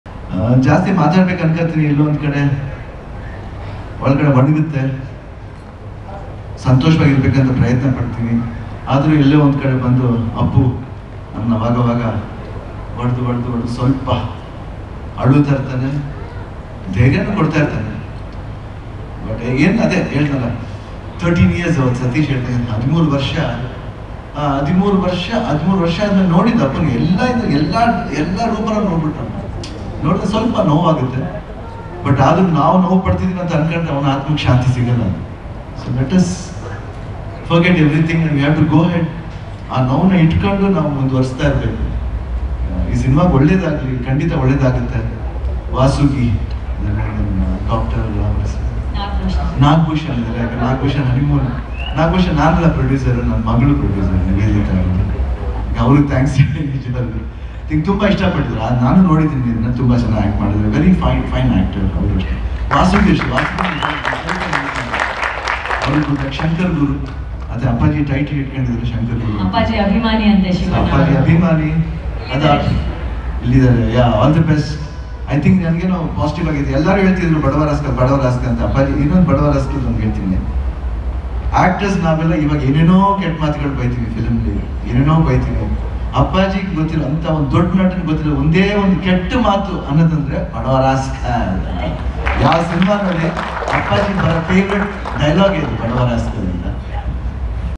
ಬಡವ ರಾಸ್ಕಲ್ ಪ್ರೀ ರಿಲೀಸ್ ಈವೆಂಟ್‌ನಲ್ಲಿ ಅಪ್ಪುರನ್ನು ನೆನೆದು ಭಾವುಕರಾದ ಶಿವಣ್ಣ | Puneeth Rajkumar
ಡಾಲಿ ಧನಂಜಯ್ ಅಭಿನಯದ ಬಡವ ರಾಸ್ಕಲ್ ಚಿತ್ರ ರಿಲೀಸ್‌ಗೆ ಸಜ್ಜಾಗಿದೆ. ಇದೇ ಹಿನ್ನೆಲೆ ಚಿತ್ರತಂಡ ಪ್ರೀ ರಿಲೀಸ್ ಈವೆಂಟ್ ಹಮ್ಮಿಕೊಂಡಿತ್ತು. ಶಿವಣ್ಣ ಕೂಡ ಕಾರ್ಯಕ್ರಮಕ್ಕೆ ಆಗಮಿಸಿ ಪುನೀತ್ ಬಗ್ಗೆ ಮಾತನಾಡಿ ಕಂಬನಿ ಮಿಡಿದಿದ್ದಾರೆ.

Actor shivarajkumar talKs about puneeth rajkumar at badava rascal pre release event